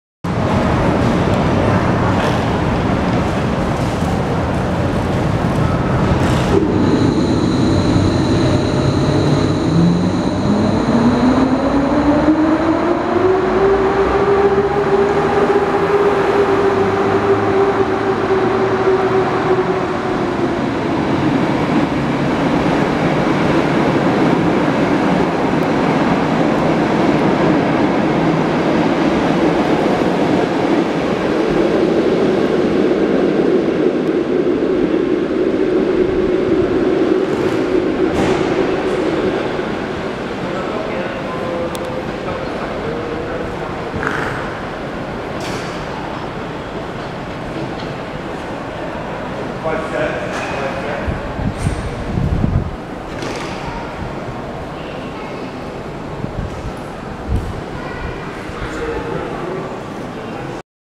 EP07-1035 (EU: 140 028-8) z TLK 45107 "BESKiDY" (B.B.GŁ.-BYDG.GŁ.) - start z WR.GŁ.
EP07-1035 (ex. EU07-050 (EU: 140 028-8)) z TLK 45107 "BESKiDY"
(BiELSKO BiAŁA GŁÓWNA  - BYDGOSZCZ GŁÓWNA)
startuje planowo z toru 1, przy peronie 1 na stacji WROCŁAW GŁÓWNY.

Adres: ul.Piłsudskiego 105, 50-085 Wrocław
(osiedle: "Przedmieście Świdnickie" na dzielnicy: "Stare Miasto"),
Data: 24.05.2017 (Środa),
Godzina: 19:21.